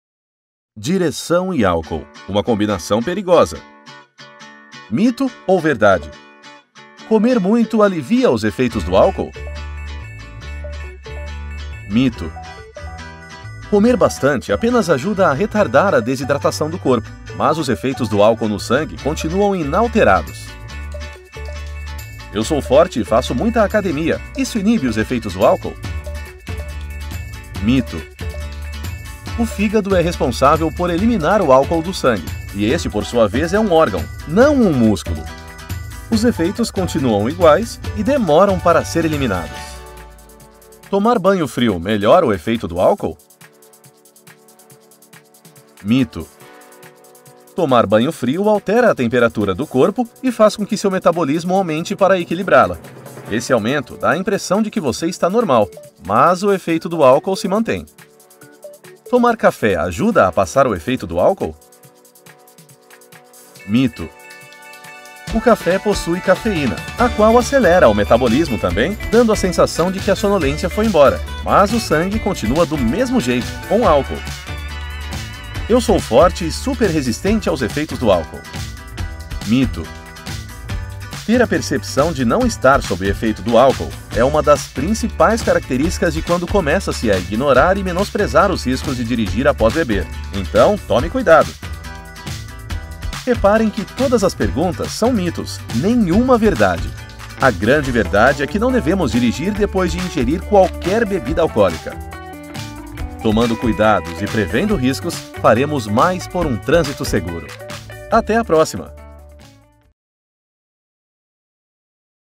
Direção e Álcool - Prefeitura de Indaiatuba
Assista e o vídeo e confira se você também acreditava em algum desses MITOS quando o assunto é ingerir álcool. Marque um amigo para ele se informar também!

Lembre-se: direção e álcool é uma combinação perigosa. Seja consciente!